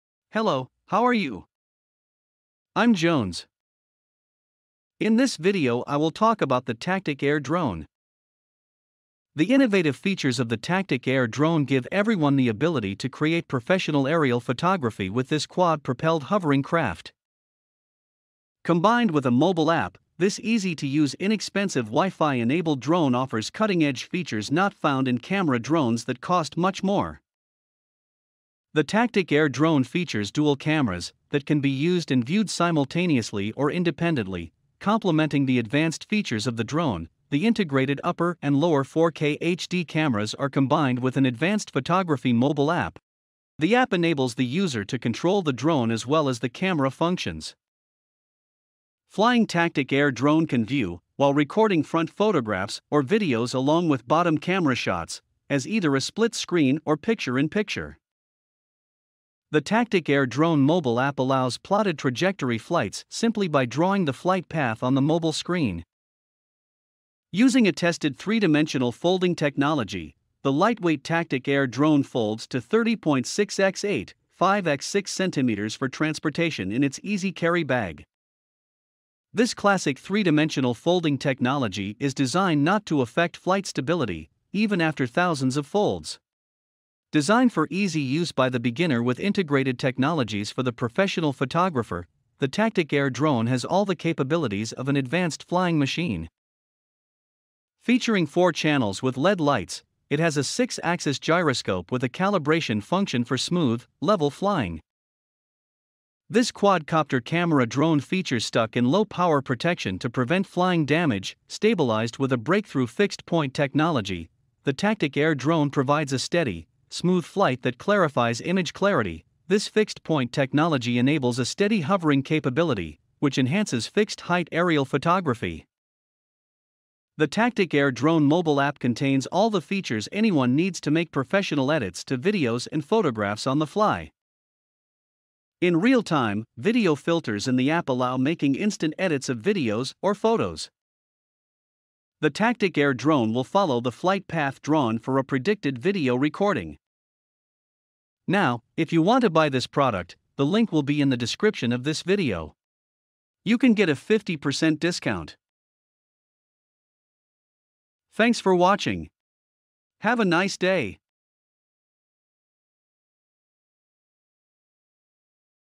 Tactic Air Drone Review ⚠️YOU NEED TO KNOW THIS ABOUT Tactic Air! Tactic Air Drone Reviews!
Tactic Air Drone features a sleek design and powerful engines for agility in the air.
This remote control drone is easy to operate, with one button to take off, one to descend, and another to come to a quick stop to prevent collisions or avoid obstacles.

=== Tactic Air Drone Features and Benefits ===

** Trajectory Mode.
** Gesture Control.
** Dual Camera.
** Battery Life.
** Camera Stabilization.